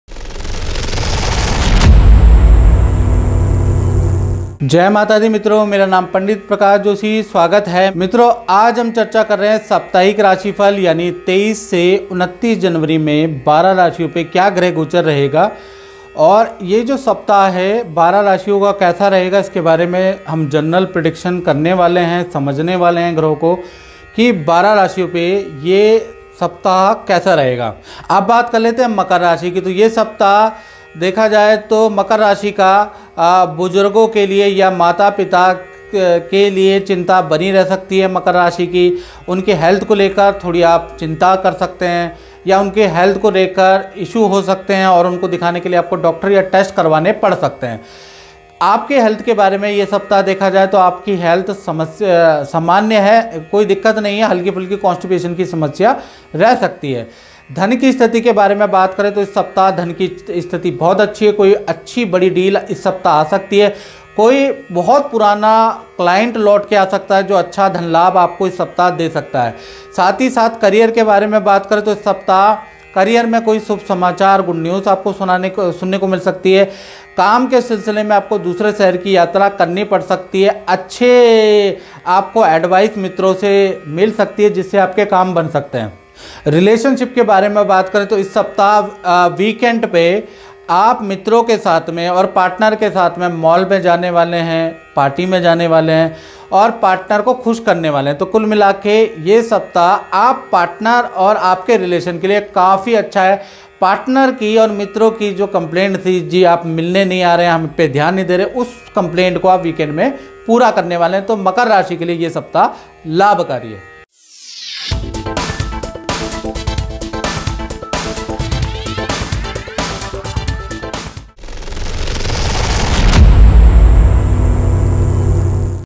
मकर राशि वालों करियर में मिलेगी सफलता | 23 से 29 जनवरी 2024 | Pt. Prakash Joshi
Thanks.